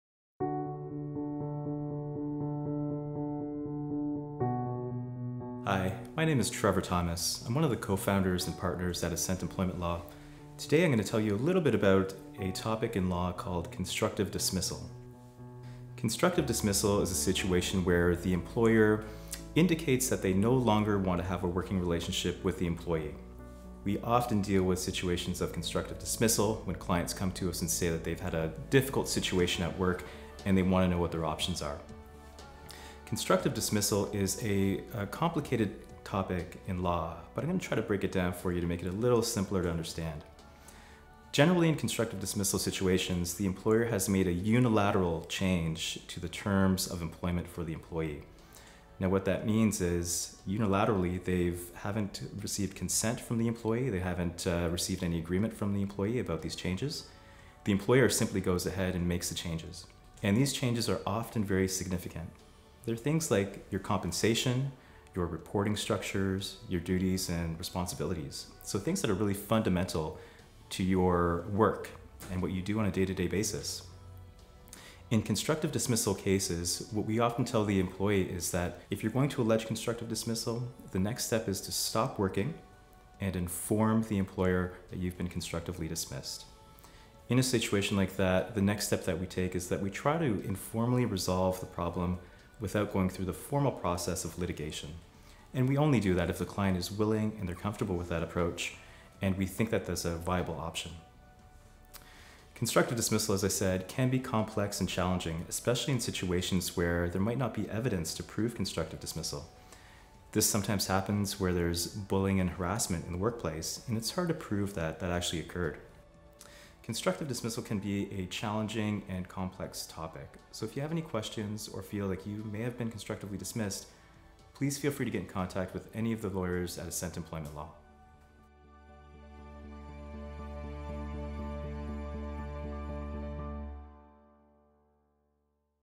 Constructive Dismissal Ascent Employment Law Trevor Thomas
What is constructive dismissal and how does it affect you? Learn here in this brief video from Ascent Employment Law Co-Founder + Partner, Trevor Thomas